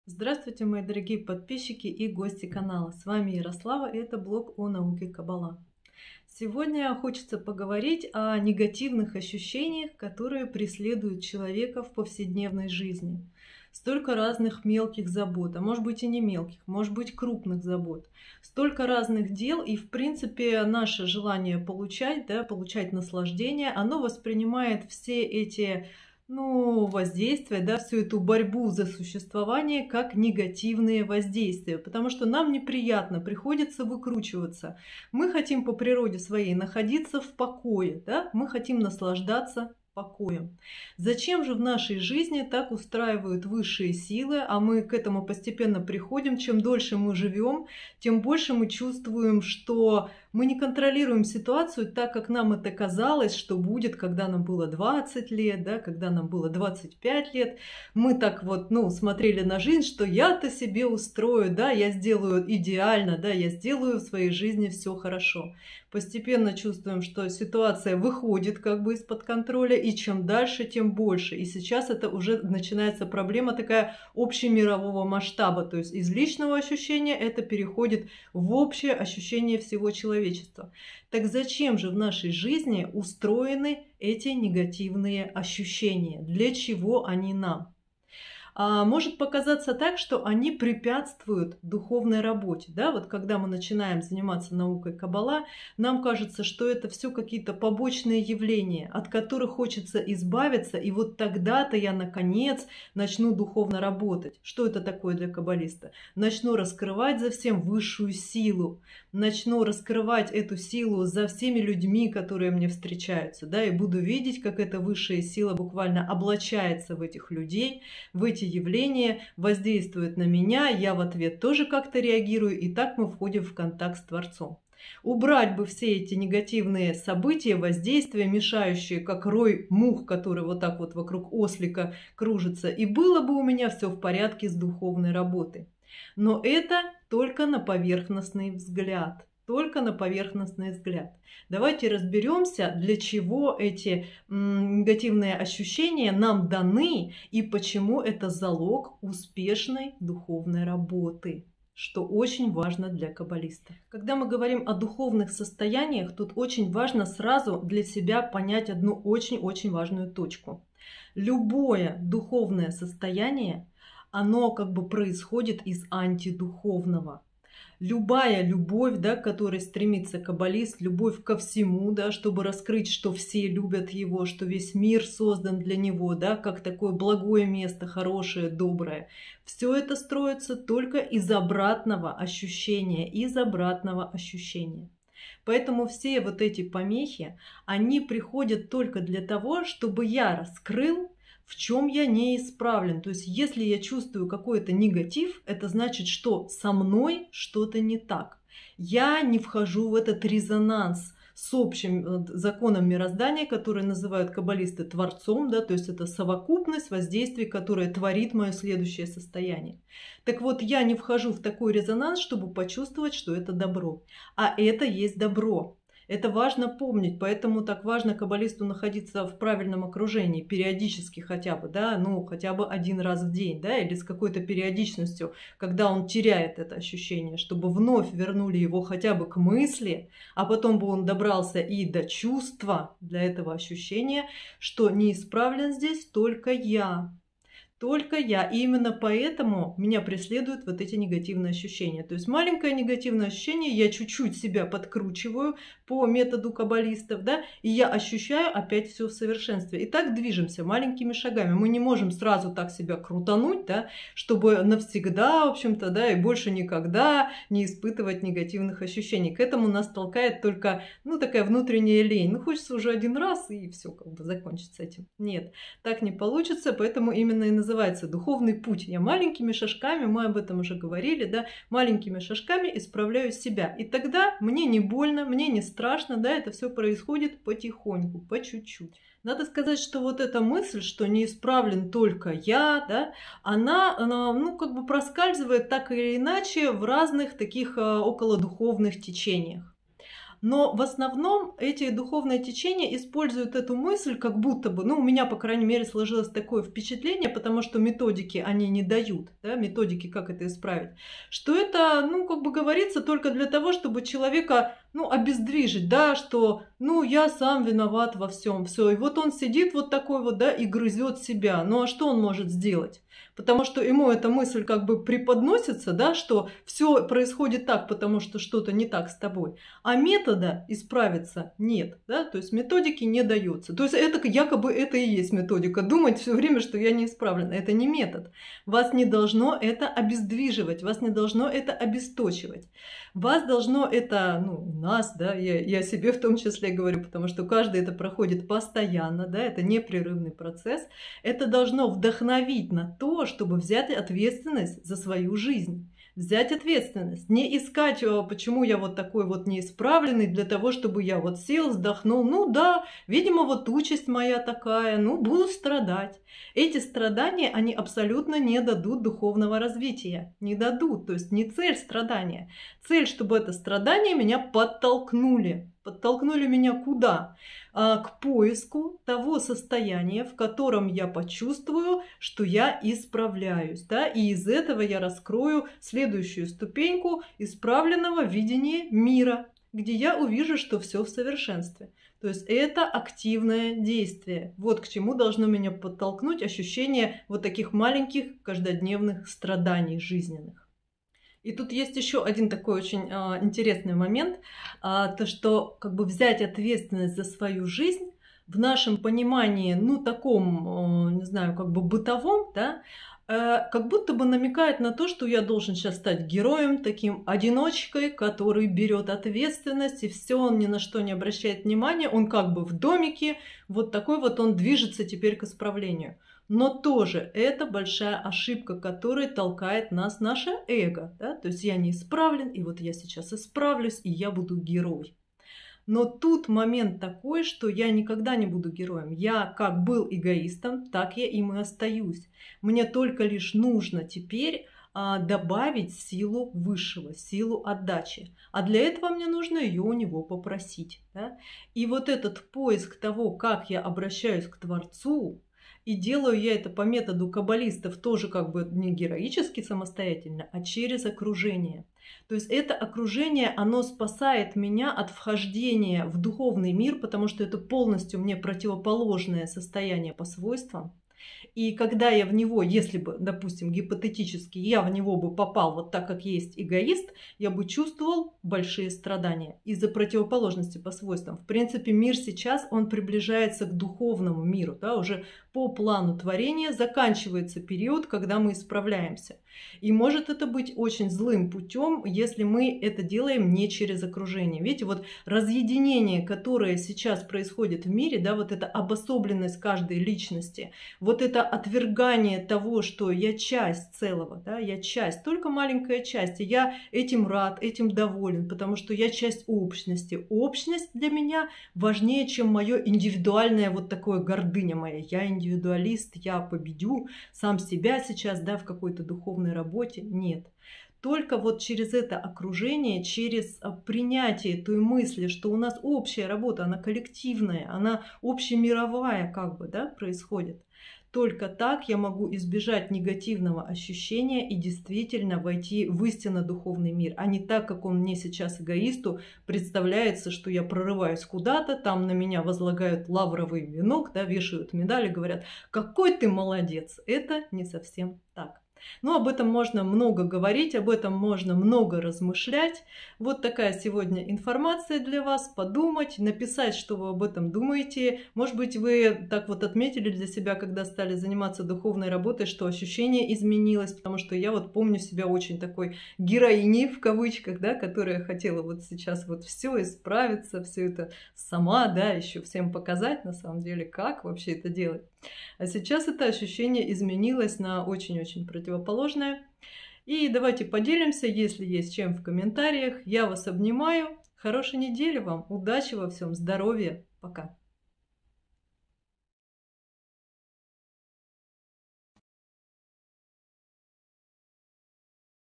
КАК УБРАТЬ НЕГАТИВ ИЗ СВОЕЙ ЖИЗНИ? Наука каббала: разум и чувства. Влог Ярославы /#324/
О чем "говорят" человеку жизненные неурядицы? Можно ли исправить ситуацию?